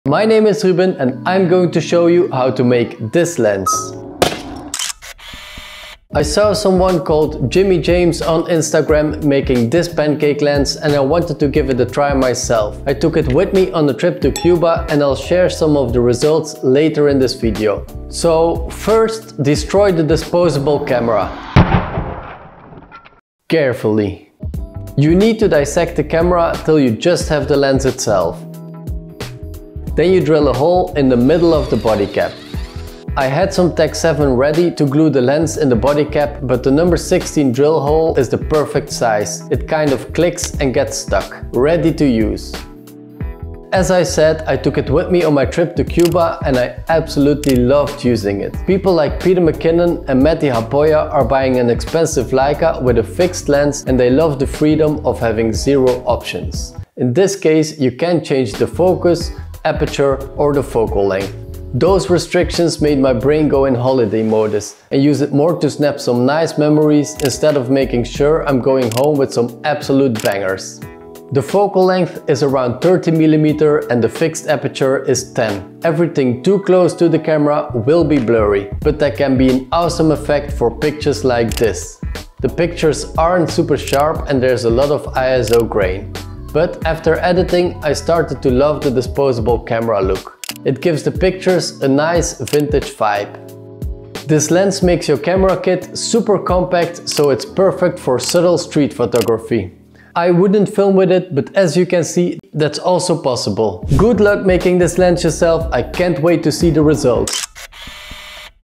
I Love This DIY Pancake Lens
In a reel from Jimi James I saw someone making a pancake lens out of a disposable camera. Of course I had to give that a try myself!

- Kodak Funsaver (Amazon)
- Sony Camera Cap (Amazon)
- Sledgehammer (Amazon)

*My camera & lens:*
- Sony fX3
- TAMRON 28-75mm

The music in this video is from Epidemic Sounds.